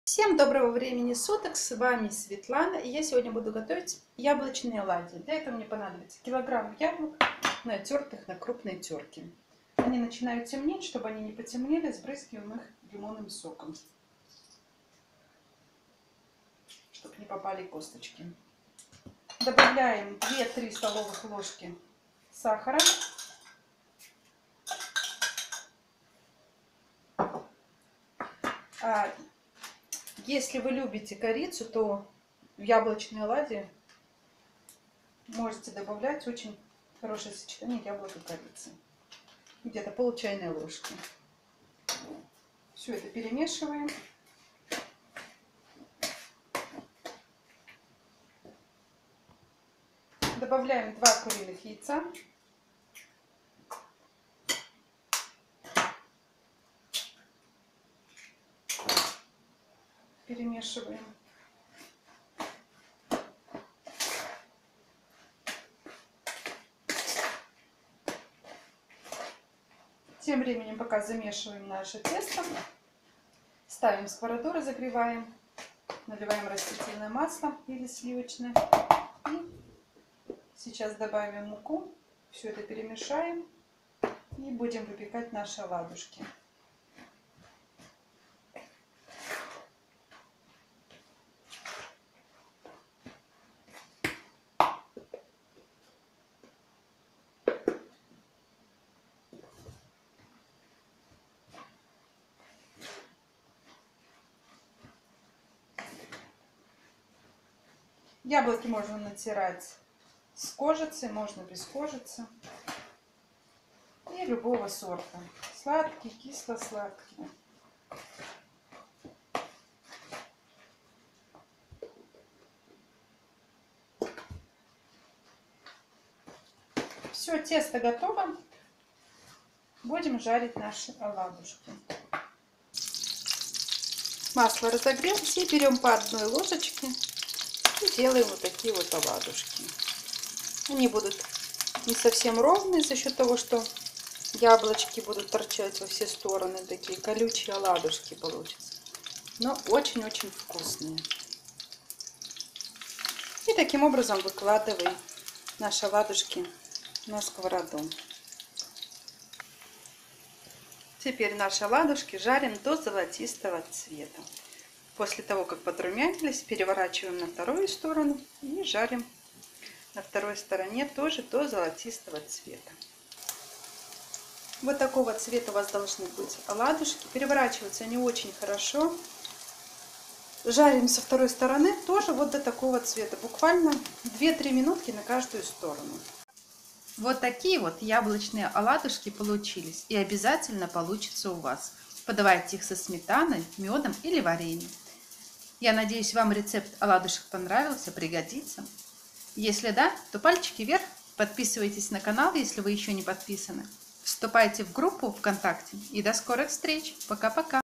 Яблочные оладьи , самый вкусный рецепт / Apple fritters
Яблочные оладьи , самый вкусный рецепт
Продукты
Яблоки - 1 КГ
Сахар - 2 -3 ст. л. (с горкой - для кислых яблок)
Мука - 3 -5 ст. л
корица по желанию
Масло подсолнечное - для жарки
НА МОЕМ КАНАЛЕ ЕСТЬ ЕЩЕ МНОГО ВКУСНЫХ и ИНТЕРЕСНЫХ РЕЦЕПТОВ.